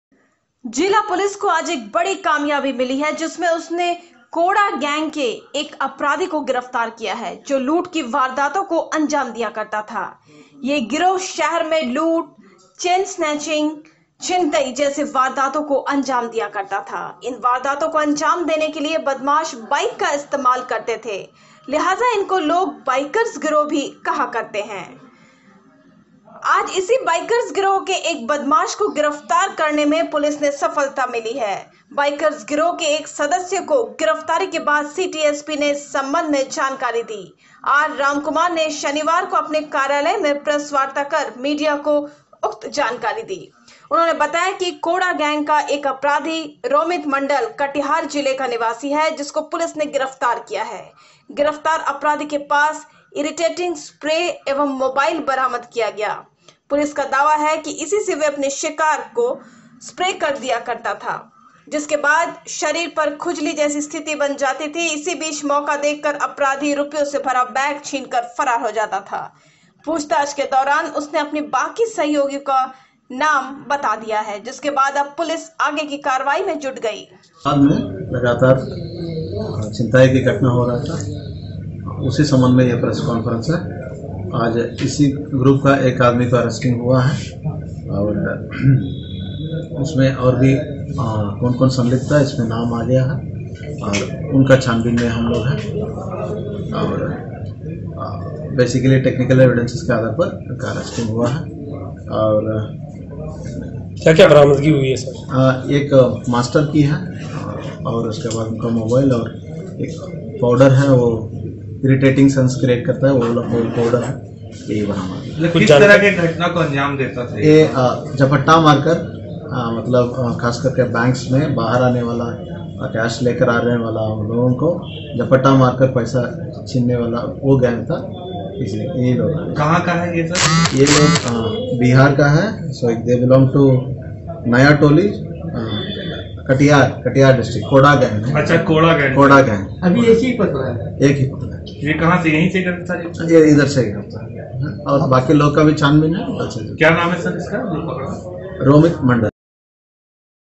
Aaj ka sach news,,13.02.2021
धनबाद : जिला पुलिस को शहर में हो रहे छिनतई मामले में बाइकर्स गिरोह के एक सदस्य को गिरफ्तार करने में सफलता मिली है। जिसके बाद सिटी एसपी आर रामकुमार ने शनिवार को अपने कार्यालय में प्रेस वार्ता कर मीडिया को बताया कि कोड़ा गैंग का एक अपराधी रोमित मंडल कटिहार निवासी है। जिसको पुलिस ने गिरफ्तार किया है। गिरफ्तार अपराधी के पास से इरिटेटिंग स्प्रे व मोबाइल बरामद किया गया है। पुलिस का दावा है कि इसी से वह अपने शिकार को स्प्रे कर दिया करता था। जिसके बाद शरीर पर खुजली जैसी स्थिति उत्पन्न हो जाती थी। इस बीच मौका देख अपराधी रुपयों से भरा बैग छीन कर फरार हो जाता था। पूछताछ के दौरान उसने अपने बाकी सहयोगियों का नाम बता दिए हैं। जिसके लिए पुलिस कार्रवाई कर रही है।
 मालूम हो कि बीते कई माह से शहर में बुजुर्ग और महिलाओं द्वारा बैंक से पैसा निकासी करने के बाद बाइकर्स झपट्टा मारकर छिनतई की घटना को अंजाम देते थे। जिससे लोगों में भय और दहशत व्याप्त हो गया था। ऐसे में कोड़ा गैंग के एक अपराधी को गिरफ्तार करने का दावा पुलिस ने किया है। देखना यह है कि इस अपराधी की गिरफ्तारी के बाद शहर में बाइकर्स गिरोह के कारनामे पर रोक लग पाती है या नहीं।